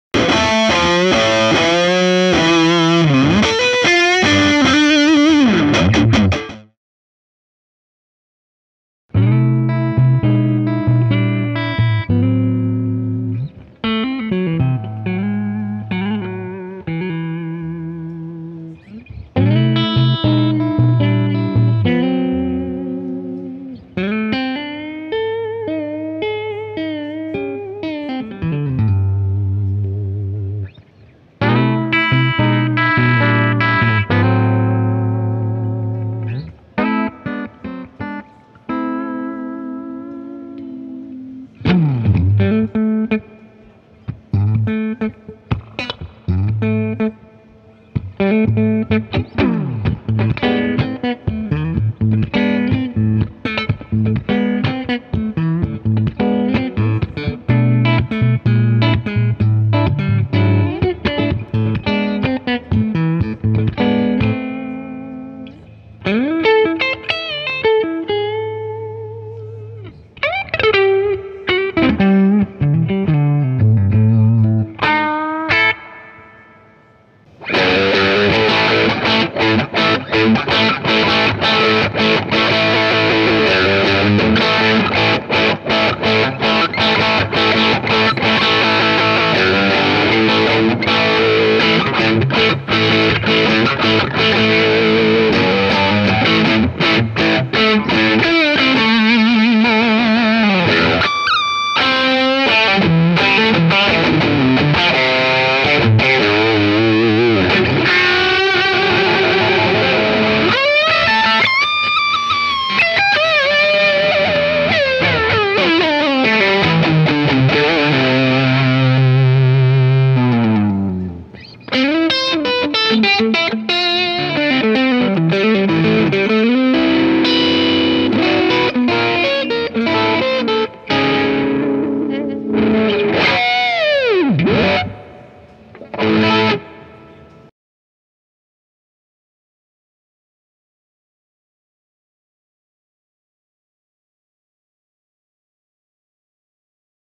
MusikMesse 2013 - Charvel Co-Cal Style 1 HH by Brice Delage
Détour par la nouvelle gamme de super Strat de chez Charvel : les Pro-Mod. Les modèles San Dimas HS et HH sont bien sûr présents dans la gamme, mais c'est à la So-Cal que nous nous intéressons aujourd'hui.